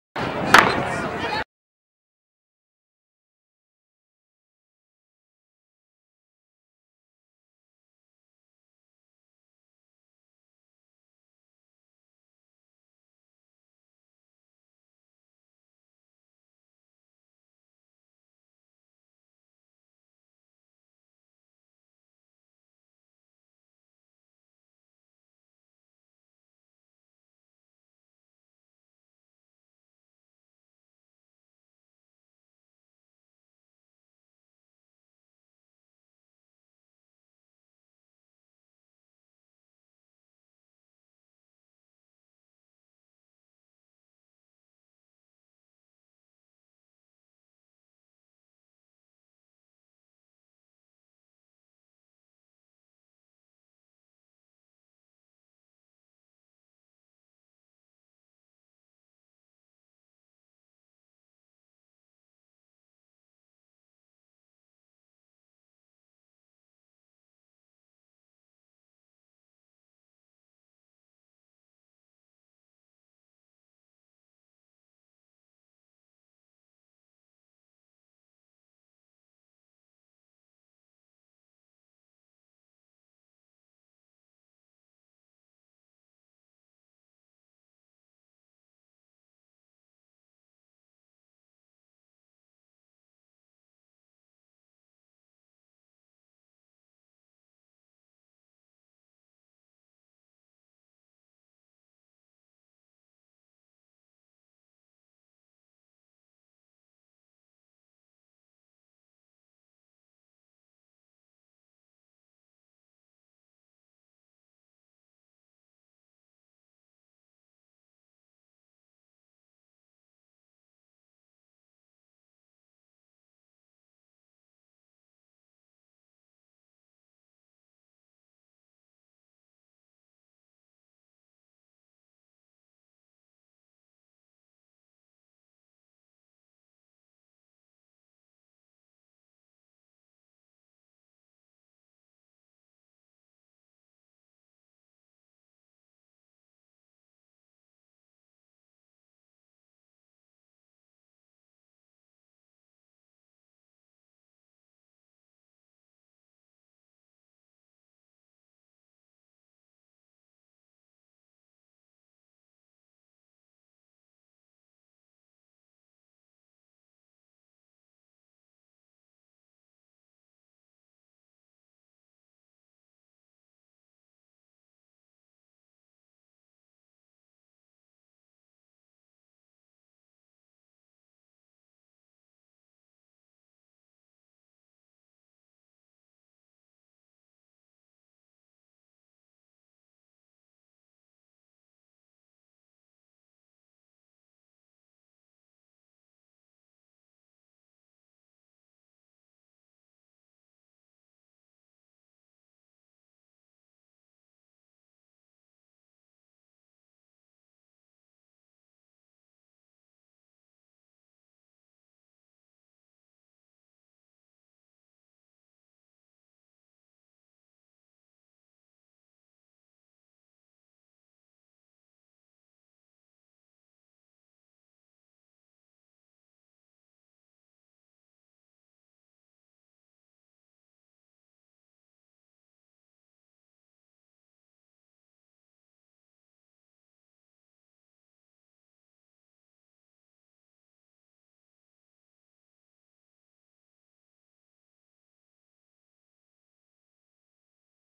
Damascus Girls 4x400 at 3A West Regional Championship
Damascus Girls 4x400 at 3A West Regional Championship at Oakdale high School